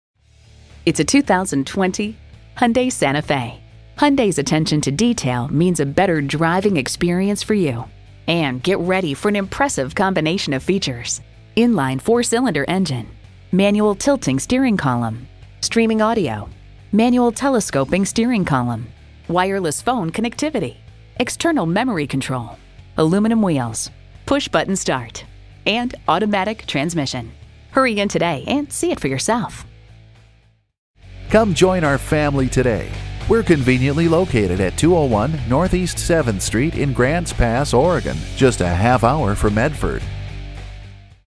2020 Hyundai Santa Fe Grants Pass OR Medford, OR #56429 - SOLD
Year: 2020
Make: Hyundai
Model: Santa Fe
Trim: SE 2.4
Engine: 2.4L I4 DGI DOHC 16V LEV3-ULEV70 185hp
Transmission: 8-Speed Automatic with SHIFTRONIC
Color: Red
Interior: Black/Black
Mileage: 30354
Stock #: 56429
VIN: 5NMS2CAD5LH177850

Address:
201 NE 7th Street